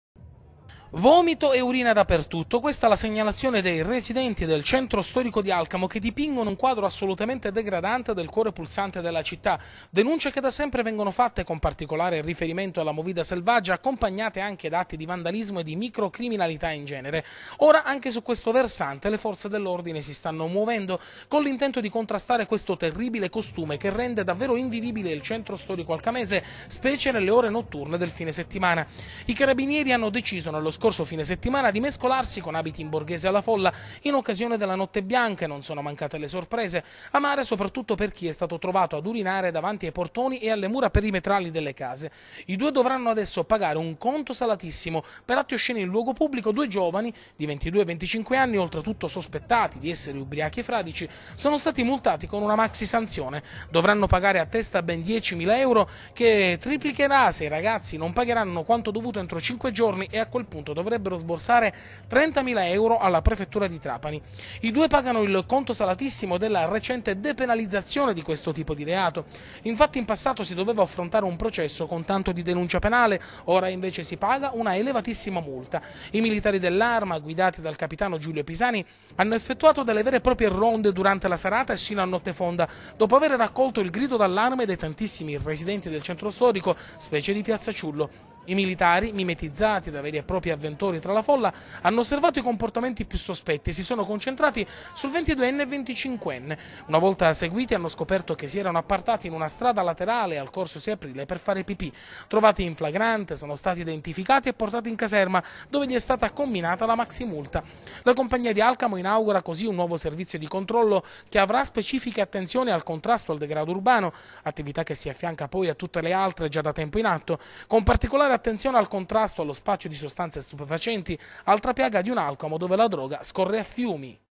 ALCAMO Urinano in strada, maxi multa da 10 mila euro per due giovani
-Urinano per strada ad Alcamo, i carabinieri li sorprendono: dovranno pagare una multa da 10 mila euro. E' accaduto a due giovani che si erano appartati per fare pipì durante la notte bianca di sabato scorso. Per loro l'accusa è di atti osceni in luogo pubblico